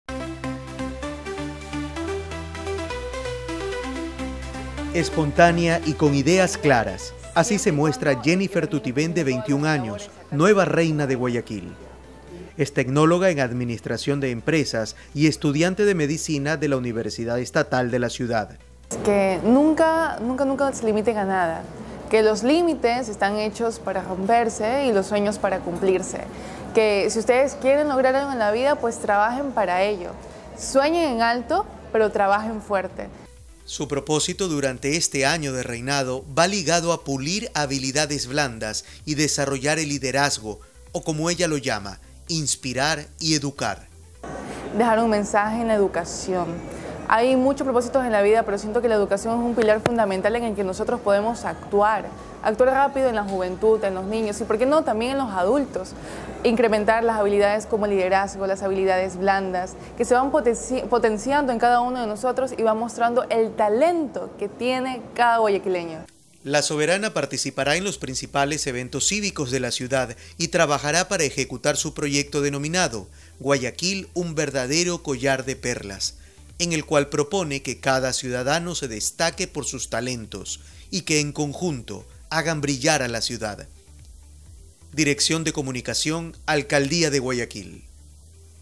Bastión Popular, cuna de la belleza
Xiomara Tutivén Campi tiene 21 años, es tecnóloga en Administración de Empresas y estudiante de Medicina. Ella es la nueva Reina de Guayaquil.

Los tres lugares preferidos de ella son: Bastión Popular, sector donde vive; la Av. 9 de Octubre, por donde desfiló y tocó el tambor mayor en fechas cívicas: y las escalinatas del cerro Santa Ana.